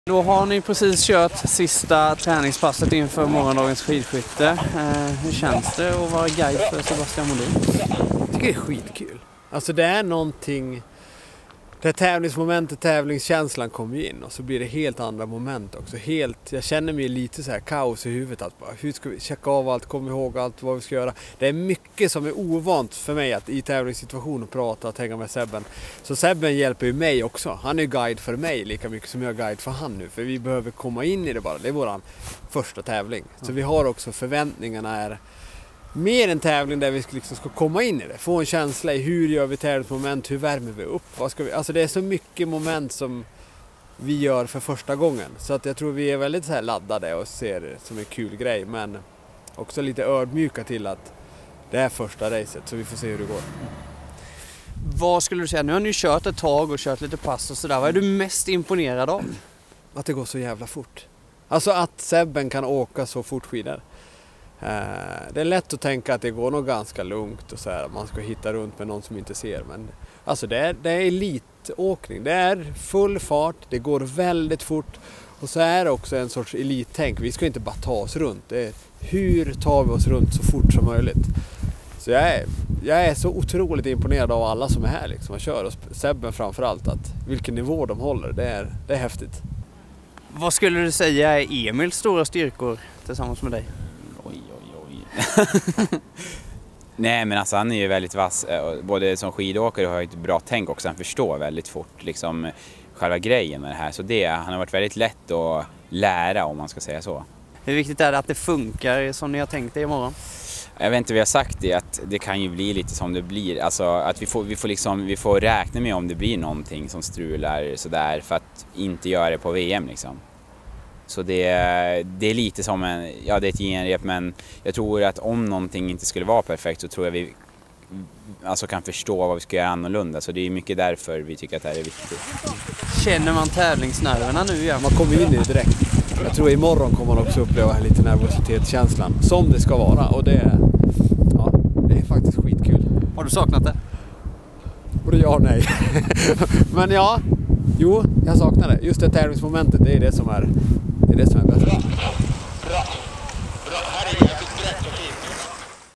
Jönsson gör debut som guide åt Modin
Emil Jönsson och Zebastian Modin ska åka VM i februari tillsammans. Exakt en månad innan mästerskapet gör de tävlingsdebut tillsammans.